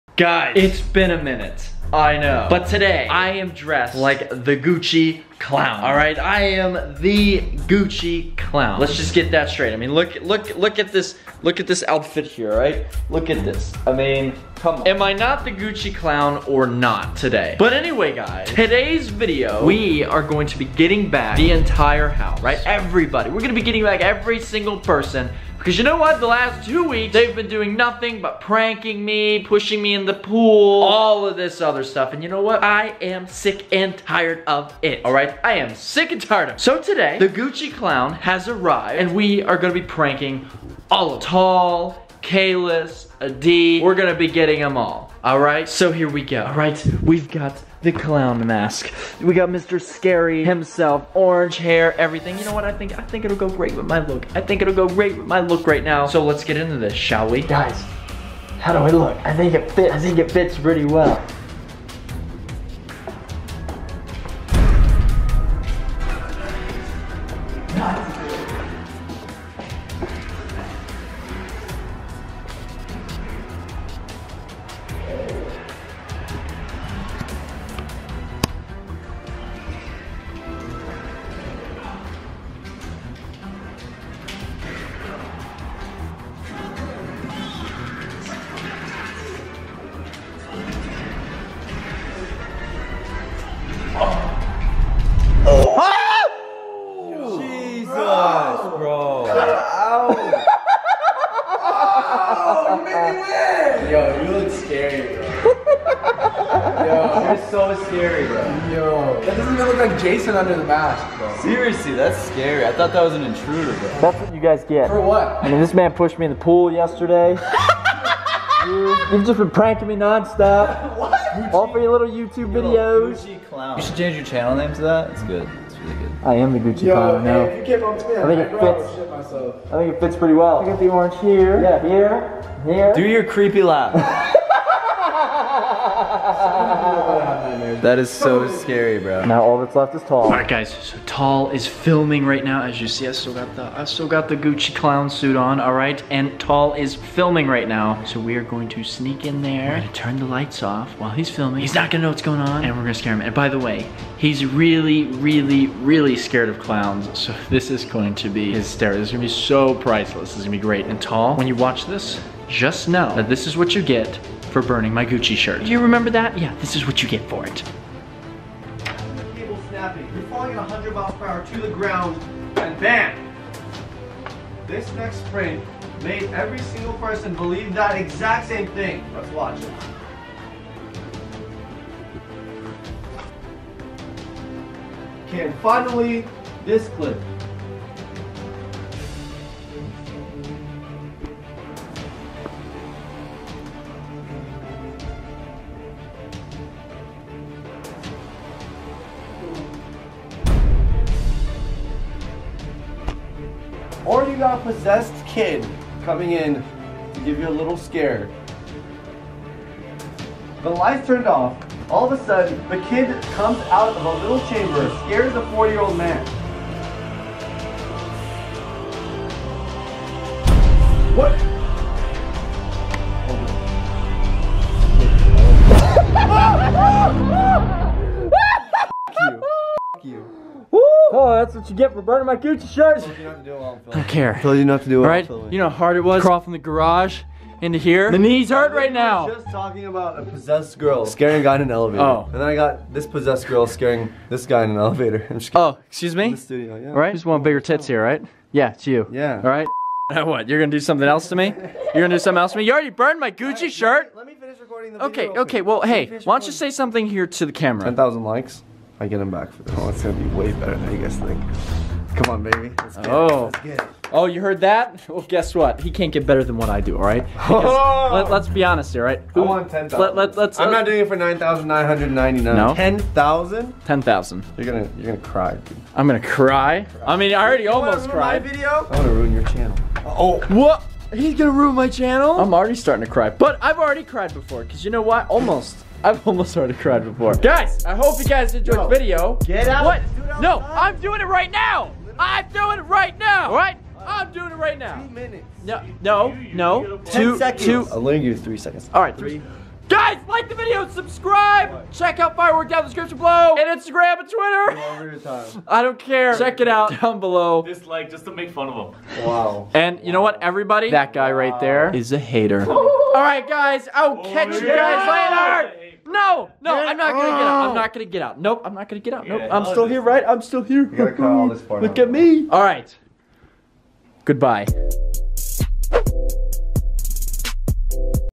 ULTIMATE Clown Scare Prank On INFINITE LISTS (FT. Reaction Time & Adi Fishman
Hope you all enjoyed this ultimate clown scare prank on my roommates Infinite Lists, Reaction Time, and Adi Fishman.